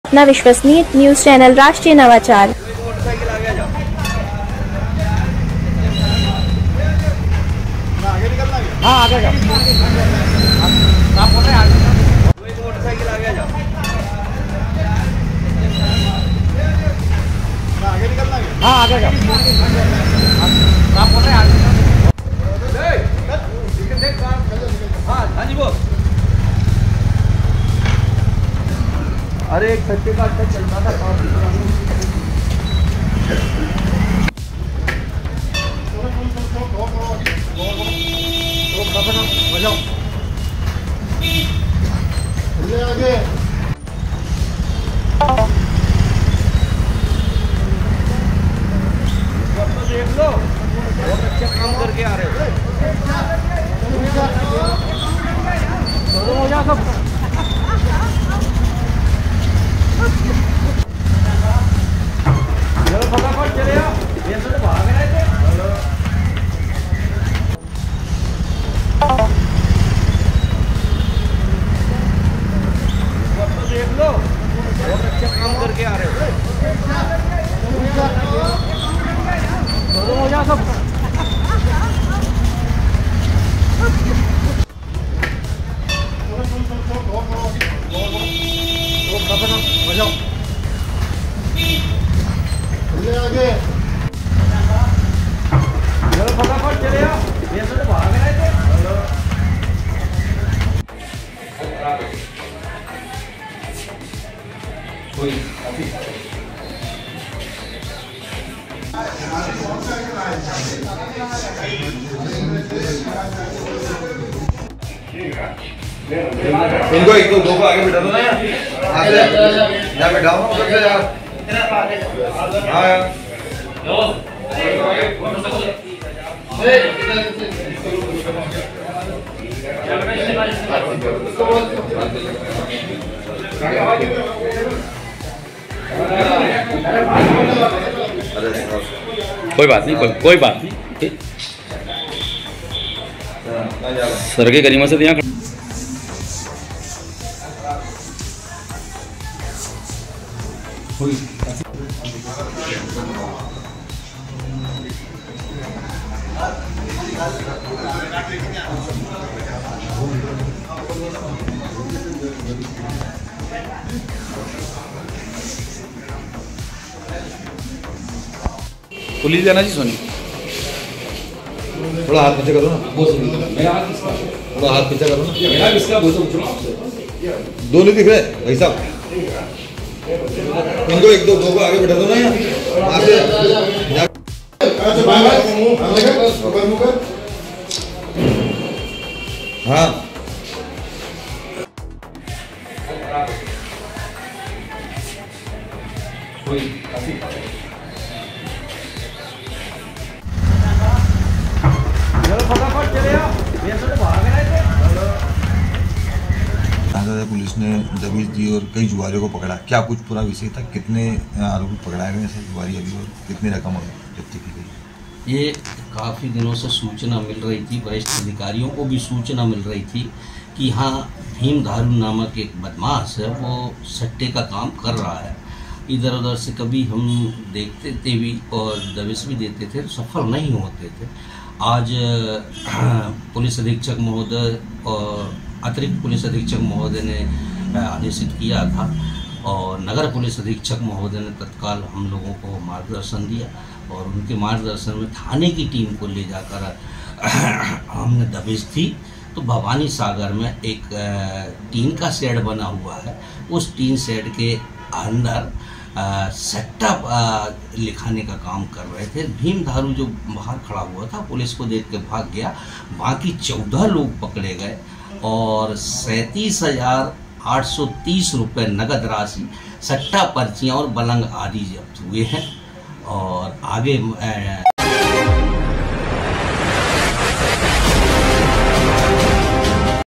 भीम धारू भाग खड़ा हुआ जब उसके जुआ सट्टे के अड्डे पुलिस ने दबिश दी। 14 बदमाश धराए, 37 हजार नकद जप्त
भीम धारू के जुआ सट्टे के अड्डे पर दबिश। पुलिस देखते ही फरार हुआ भीम धारू। नाहर दरवाजा थाना पुलिस ने भवानी सागर से 14 जुआरियों को जुआ खेलते पकड़ा। जुआरियों के पास से तास पत्ती सहित 37 हजार से ज्यादा की नगदी भी जप्त।